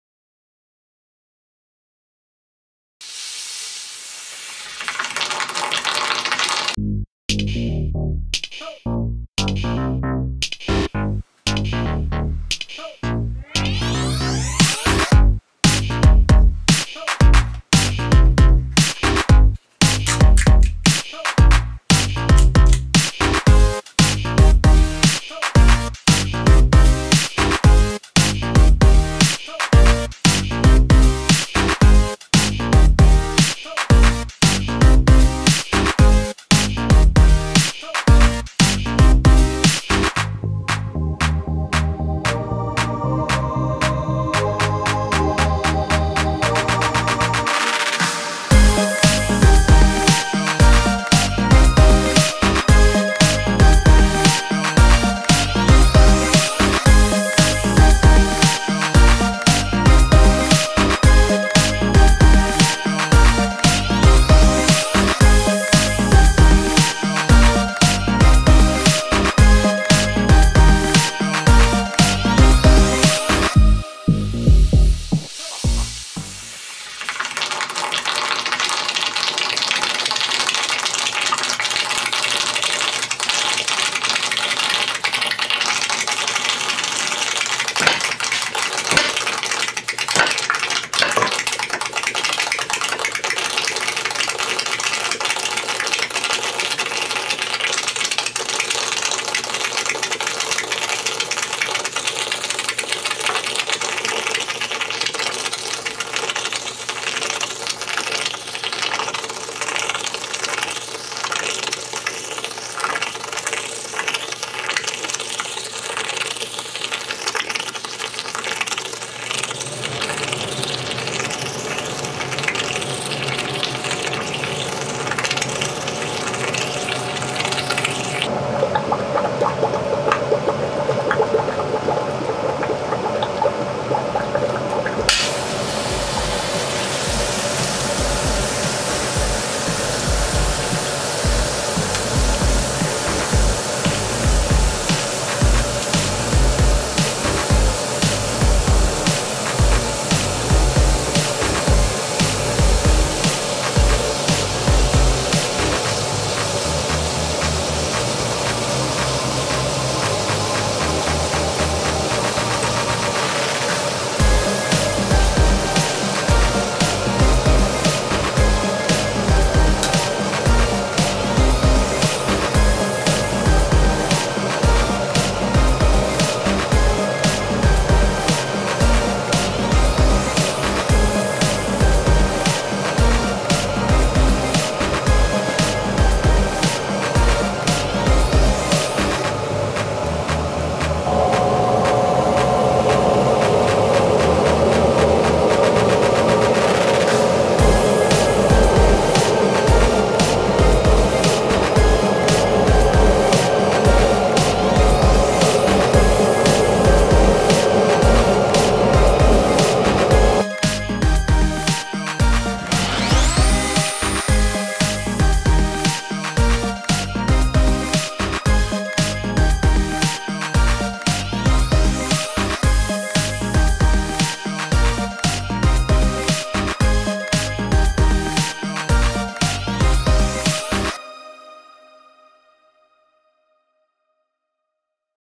Honey VS 1000 Degree Glowing Metal Ball Experiment
This is a promotional video for Inside the Onion. Check out the link for more awesome destruction videos!